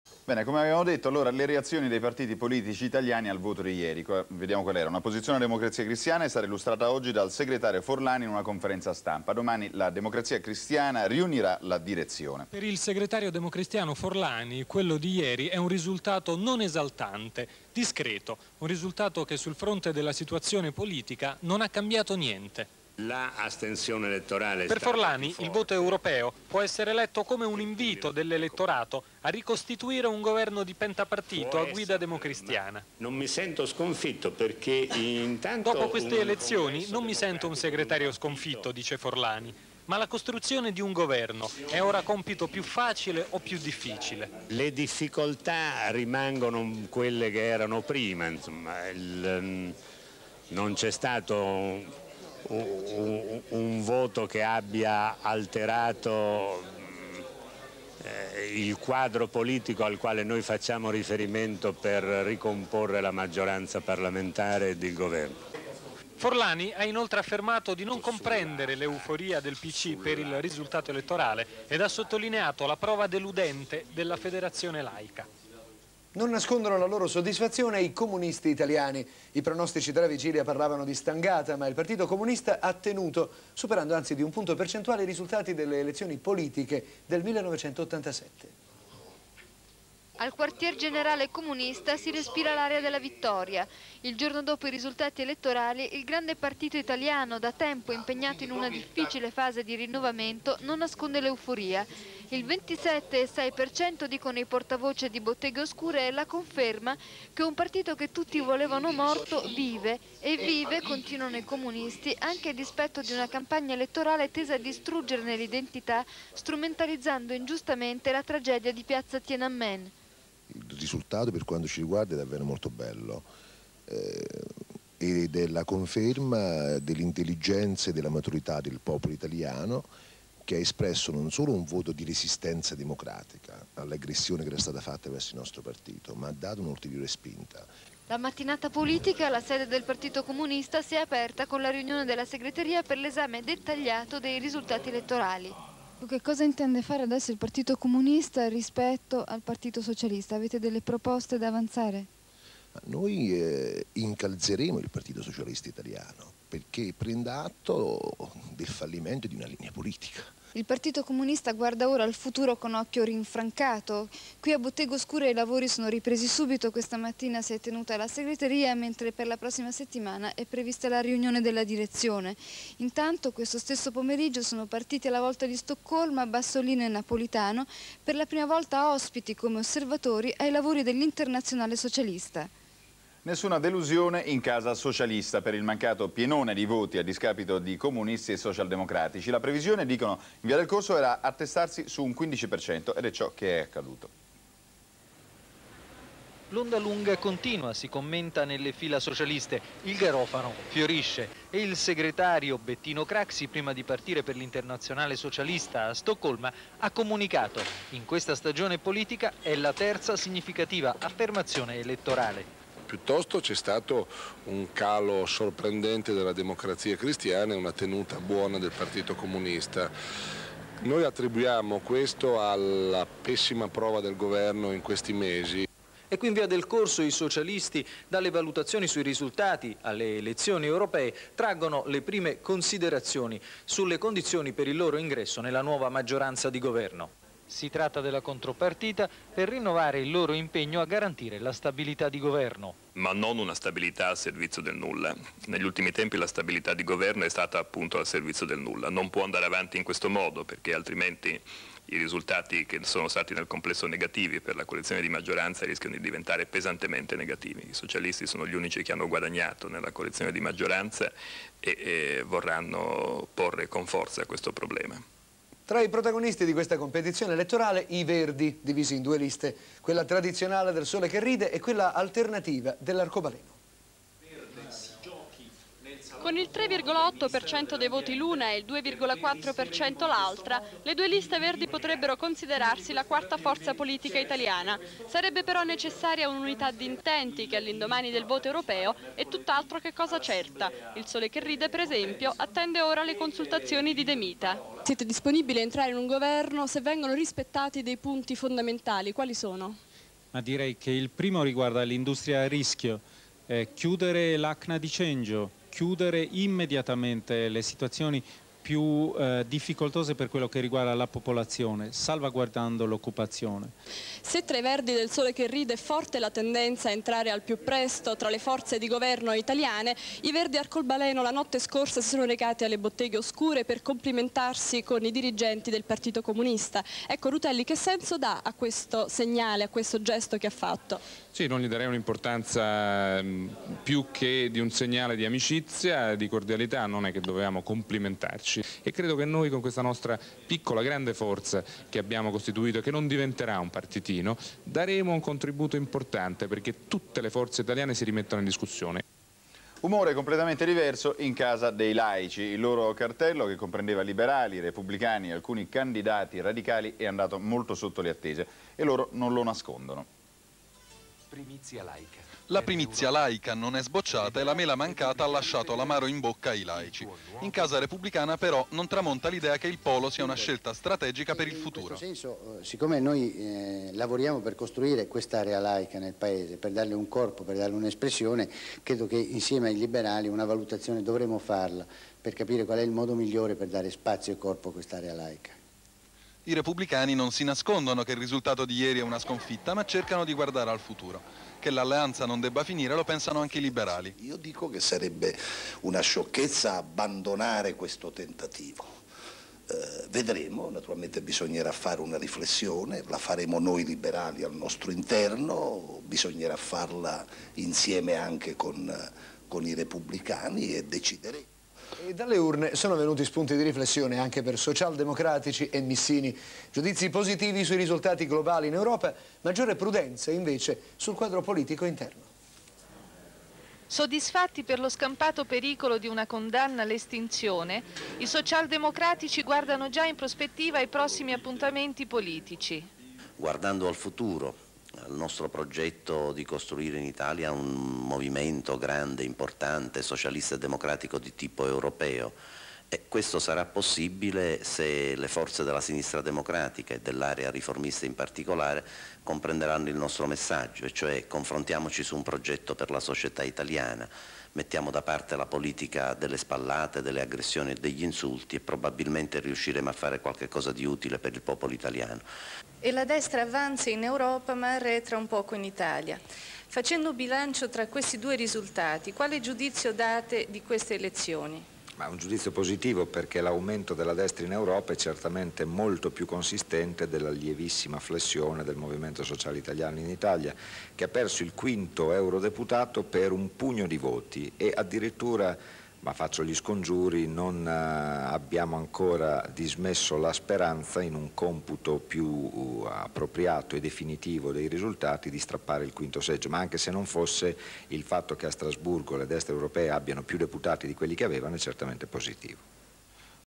Elezioni europee 1989 - Le reazioni dei partiti italiani
Nelle parole di Arnaldo Forlani, Ugo Intini, Antonio Bassolino, Gianni Mattioli, Francesco Rutelli, Giorgio Medri, Egidio Sterpa, Carlo Vizzini, Gianfranco Fini